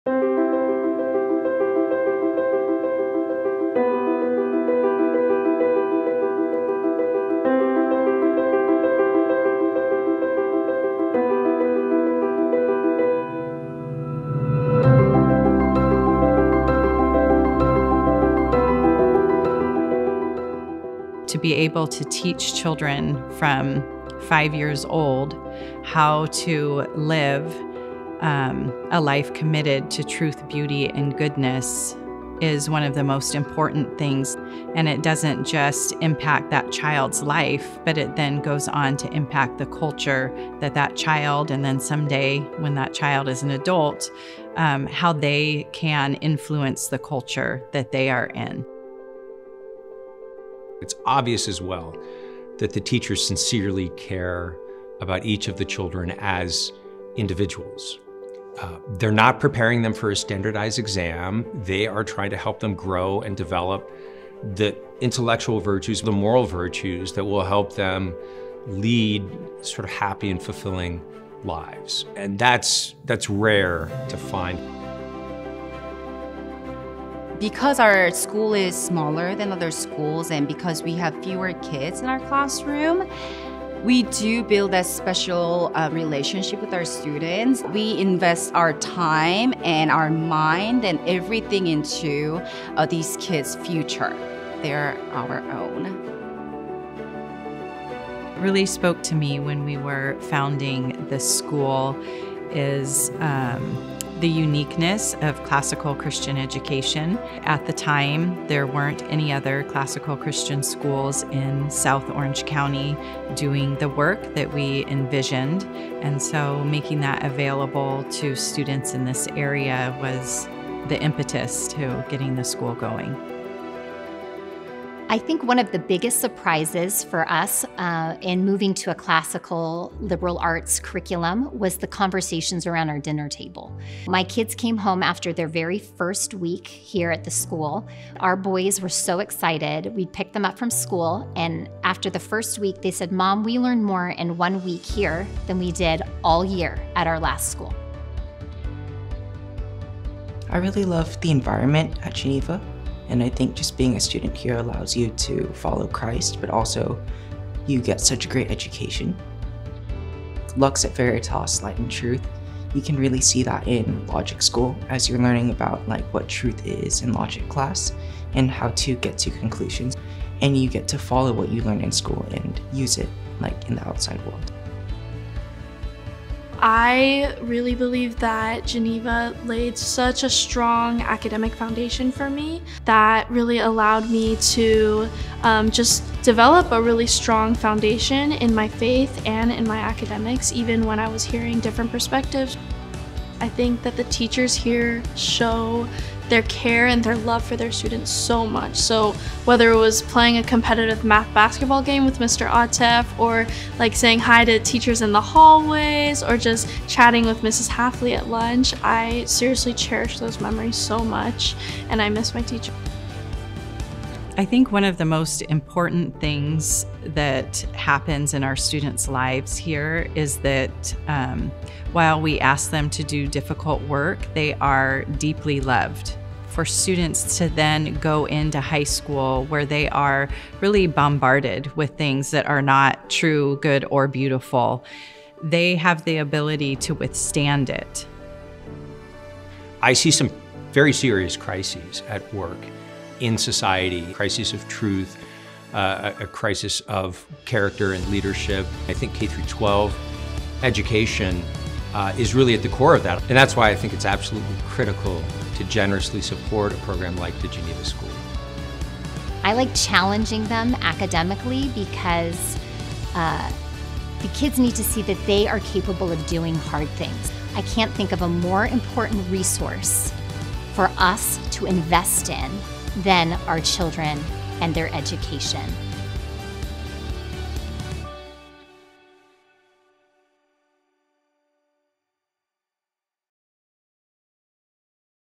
Geneva School Orange County School Video Production
Discover the exceptional educational experience offered at Geneva School through our captivating school video production. In this video, we showcase the unique curriculum, vibrant campus life, and nurturing community that define Geneva School in Orange County. From innovative teaching methods to enriching extracurricular activities, experience firsthand why Geneva School is a leader in private education. Whether you're a parent considering enrollment or a community member curious about our school's mission, this video provides a comprehensive overview of what sets Geneva School apart. Join us as we celebrate academic excellence and character development at Geneva School in Orange County.